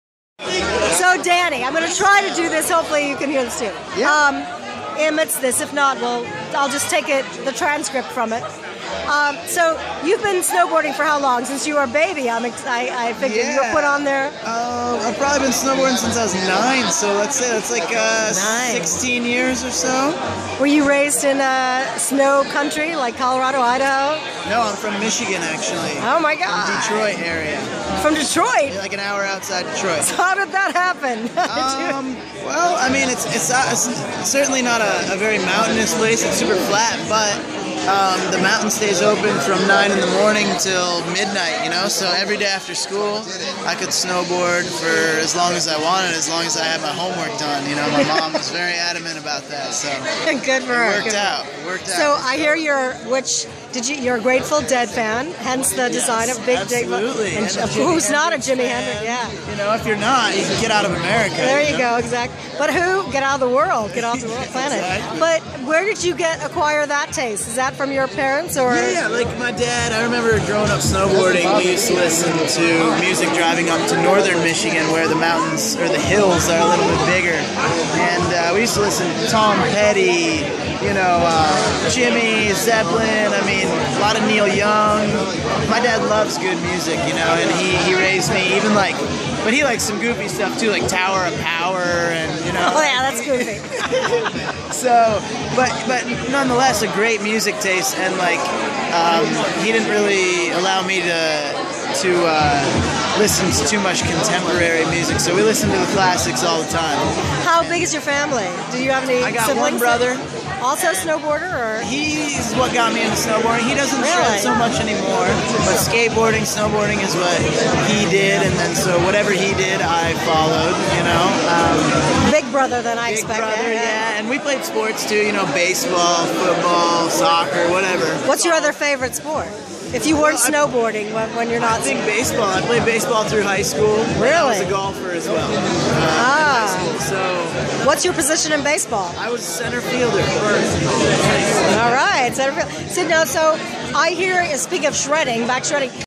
Danny Davis
Interview @Burtonsnowboard for launch of Team USA 2014 uniforms